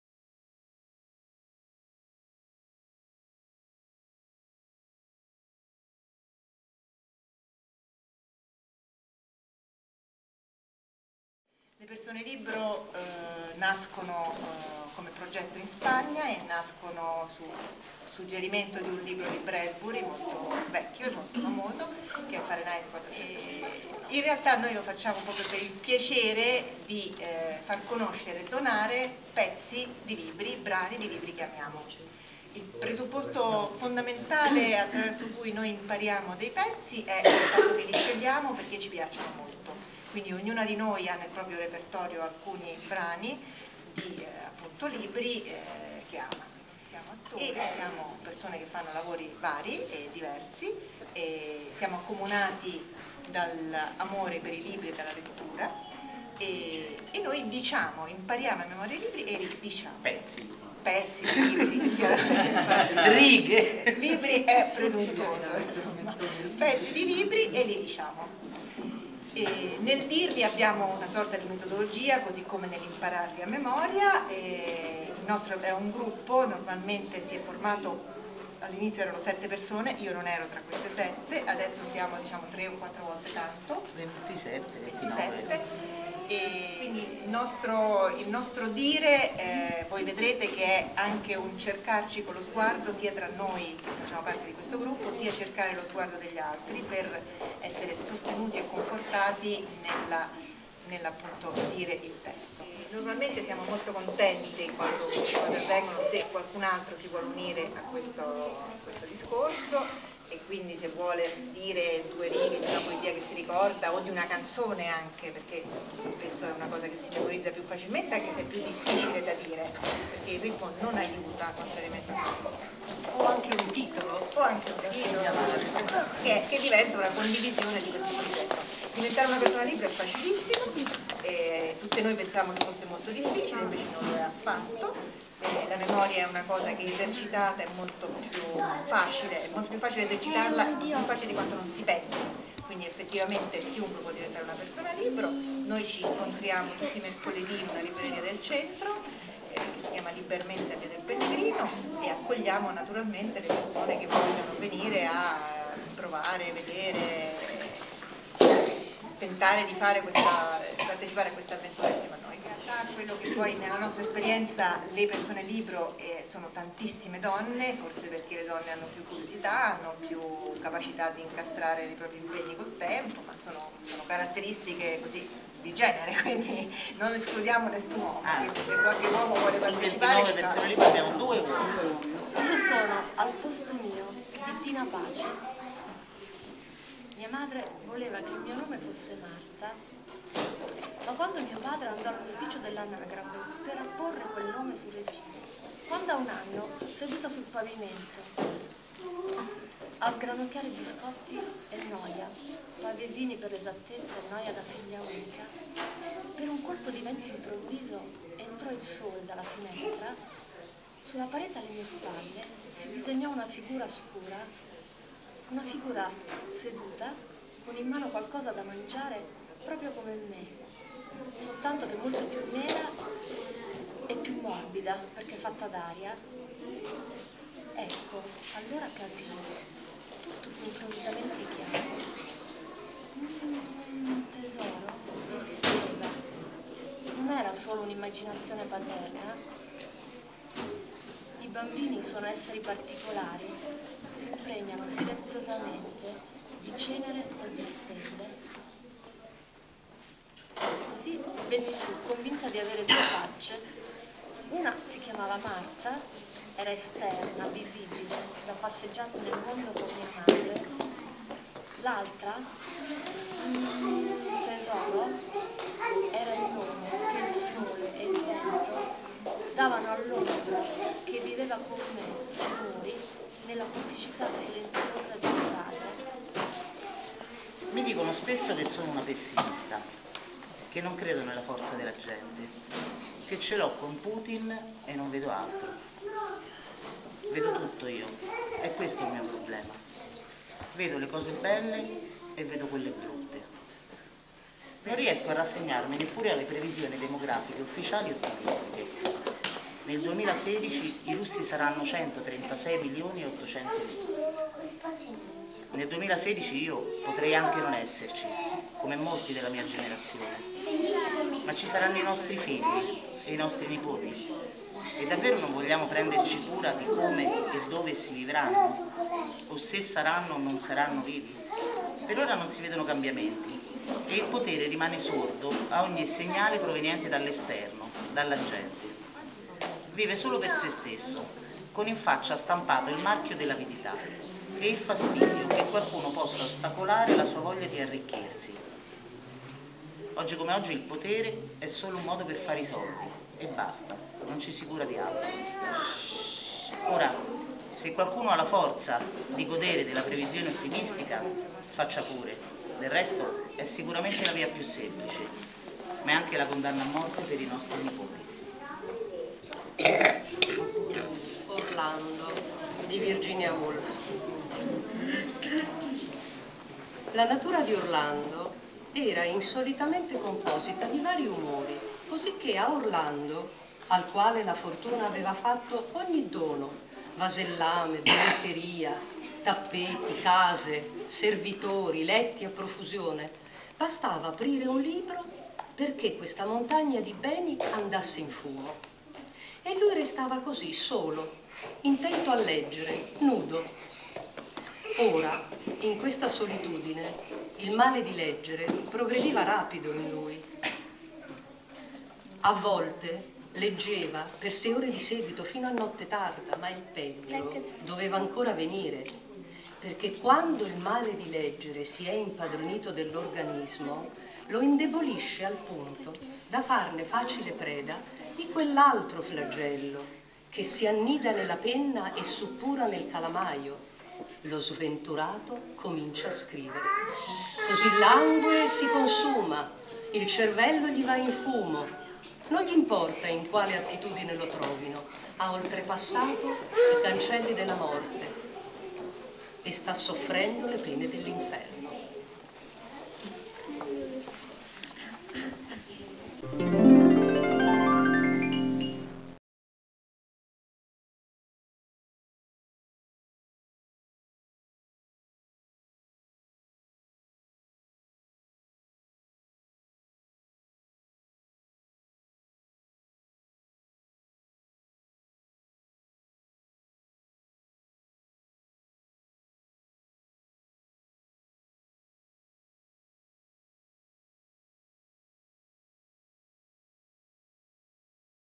Persone libro alla Scuola Iqbal Masih
Le Persone libro alla Scuola Elementare Iqbal Masih in un pomeriggio di incontro e riflessione sul futuro della scuola tra insegnanti, genitori e alunni.
In questo video i testi: Al posto mio di Tina Pace, Diario Russo di Anna Politkovskaja e Orlando di Virginia Woolf.

Le Persone libro sono lettrici e lettori che amano i libri tanto da volerli imparare a memoria: non tutti ma i pezzi, i frammenti che più li appassionano, che più sentono risuonare dentro. E vanno in giro a dirli, a donare le parole che amano a chi vuole fargli il dono altrettanto importante dell'ascolto.
A cura dell'associazione donne di carta.